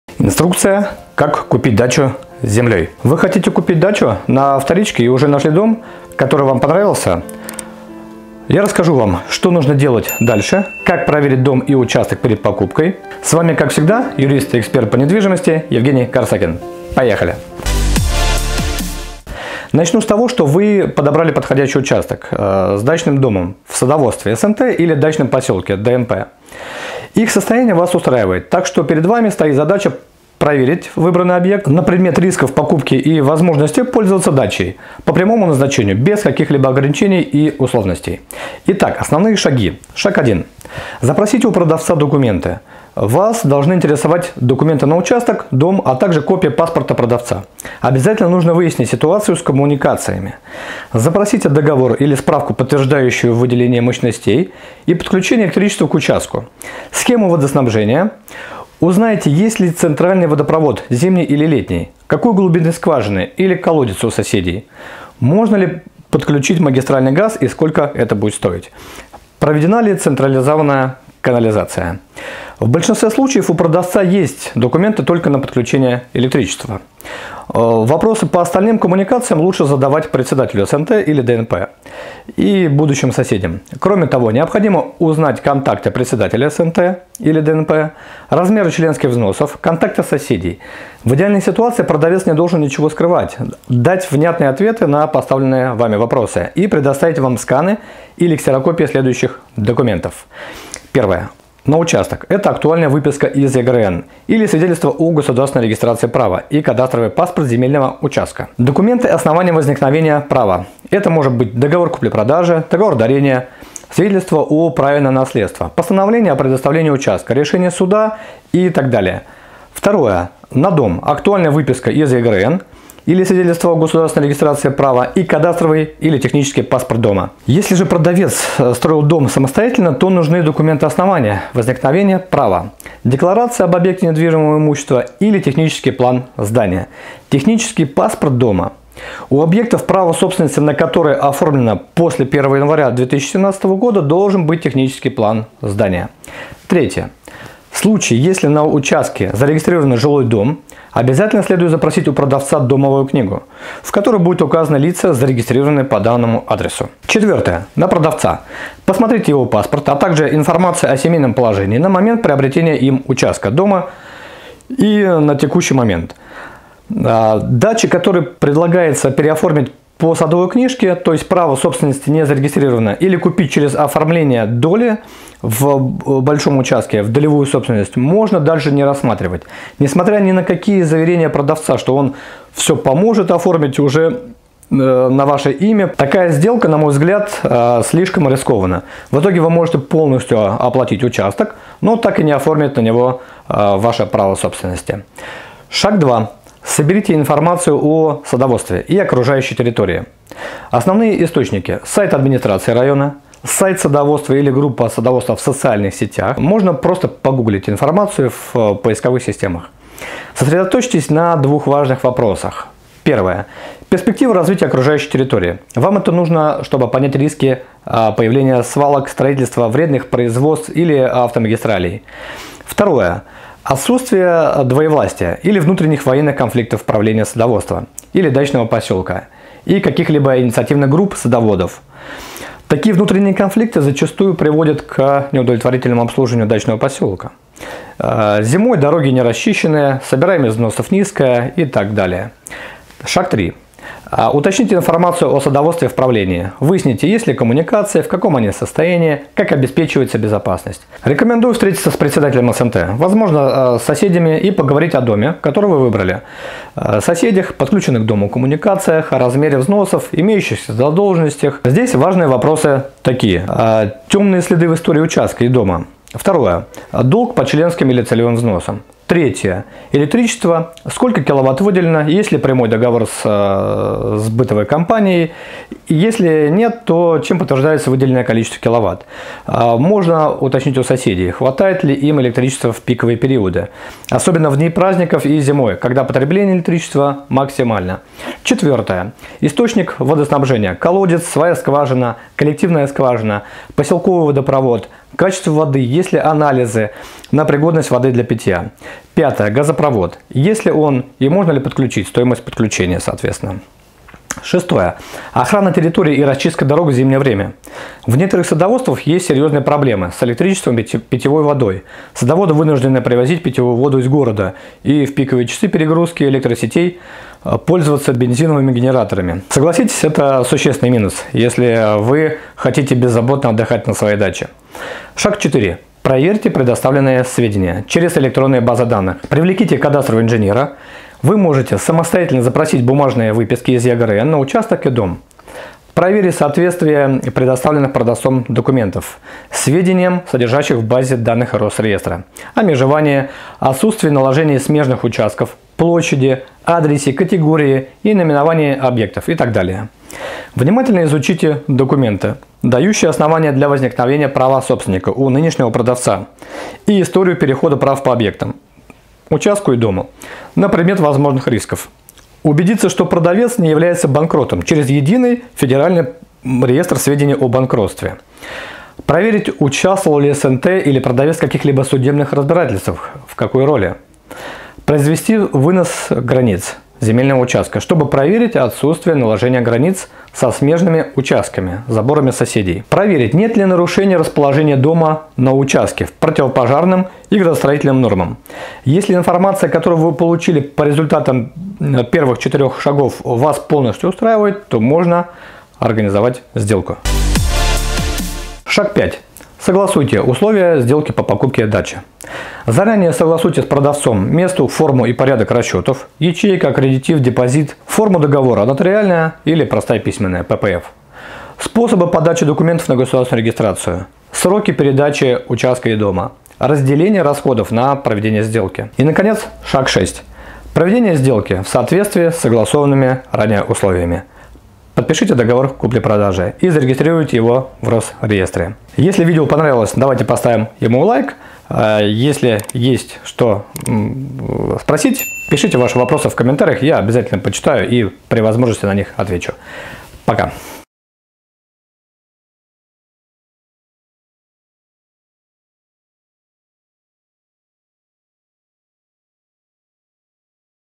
Инструкция: как купить дачу с землей?
в этом видео Вы узнаете:
👉 Как правильно купить дачу с землей? Инструкция детальная к покупке. Вы хотите купить дачу на вторичке и уже нашли дом, который вам нравится. Что нужно делать дальше? Как проверить дом и участок перед покупкой?

Шаг 1. Запросите у продавца документы.
Шаг 2. Соберите информацию о садоводстве и окружающей территории.
Шаг 3. Уточните информацию о садоводстве в правлении, выясните, есть ли коммуникации, в каком они состоянии, как обеспечивается безопасность.
Шаг 4. Проверьте предоставленные сведения через электронные базы данных и официальные запросы, привлеките кадастрового инженера.
Шаг 5. Согласуйте условия сделки по покупке дачи.
Шаг 6. Проведение сделки в соответствии с согласованными ранее условиями.
Подпишите договор купли-продажи и зарегистрируйте его в Росреестре.